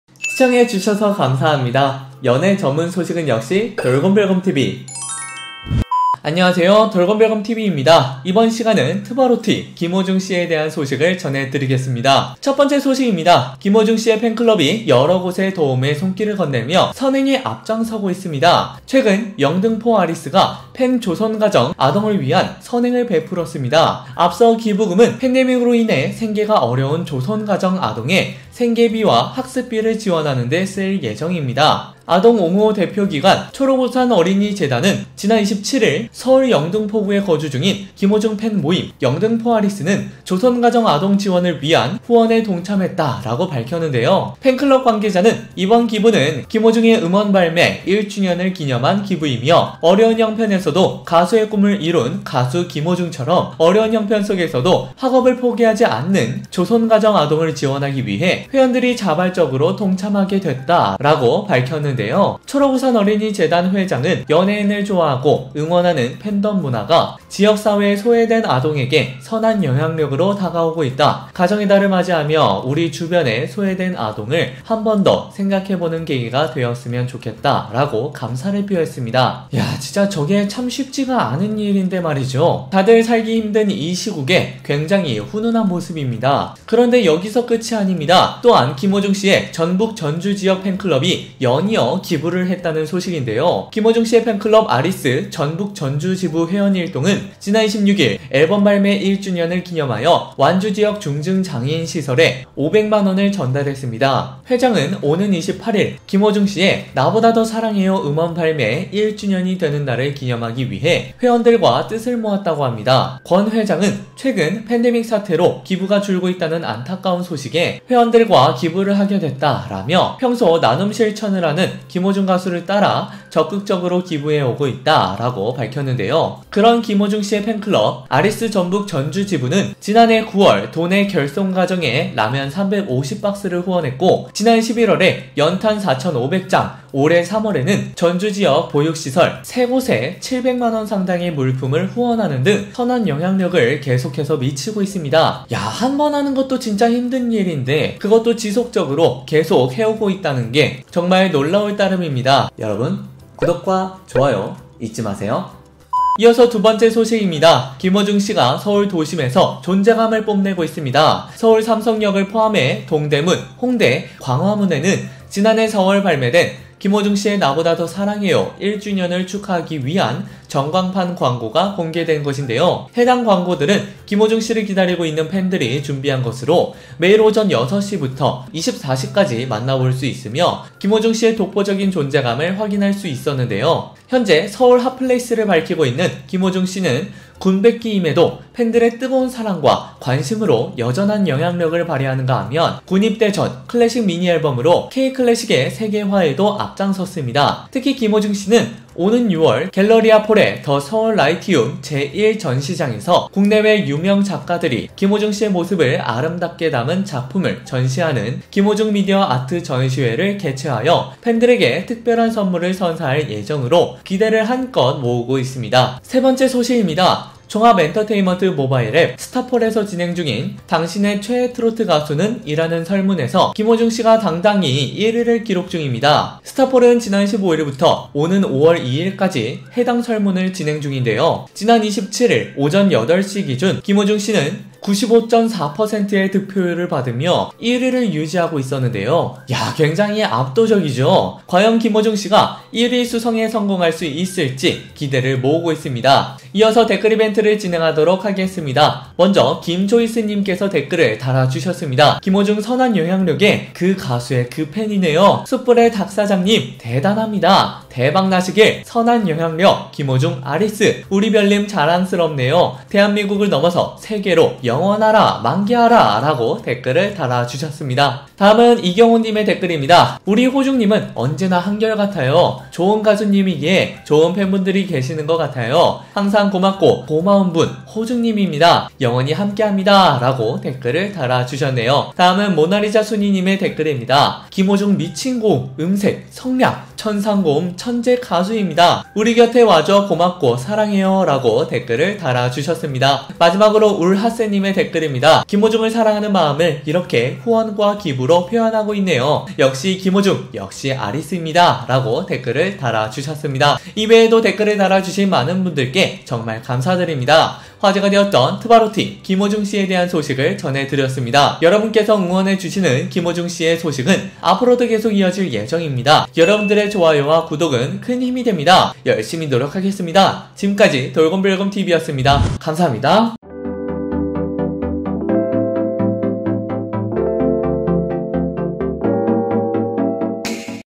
【ENG】트바로티 김호중, 선한 영향력 끝판왕!! 기부가 끊이질 않는다!!! Kim Ho-joong 돌곰별곰TV
감사합니다.

네이버 파파고 다국어 번역 :

Tvarotti Kim Ho-joong, the king of good influence! Donations never stop!!!

トゥバロッティ キム·ホジュン、善良な影響力の最高峰！ 寄付が絶えない!!!

Tvaro.T金浩中，善良的影響力終結者！ 捐款不斷！！！